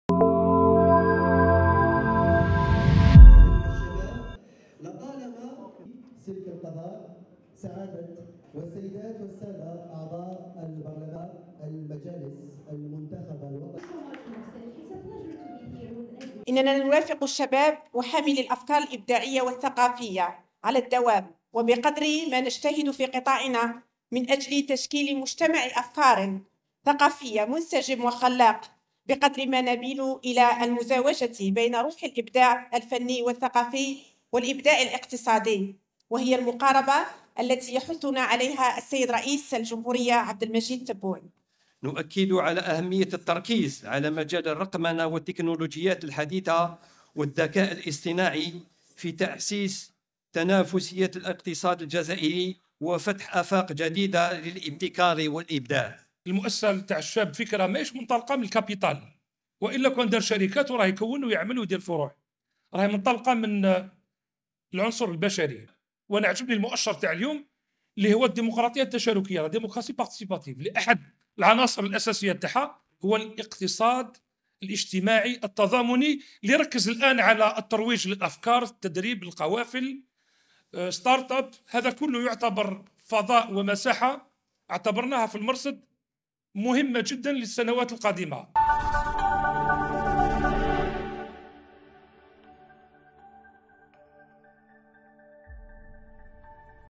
إطلاق القافلة الوطنية "شاب فكرة" في طبعتها الثالثة
الجزائر (وأج)- تم اليوم الخميس بالجزائر العاصمة, إطلاق القافلة الوطنية "شاب فكرة" في طبعتها الثالثة التي تهدف الى مرافقة الشباب حاملي الأفكار من أجل تجسيد مشاريعهم.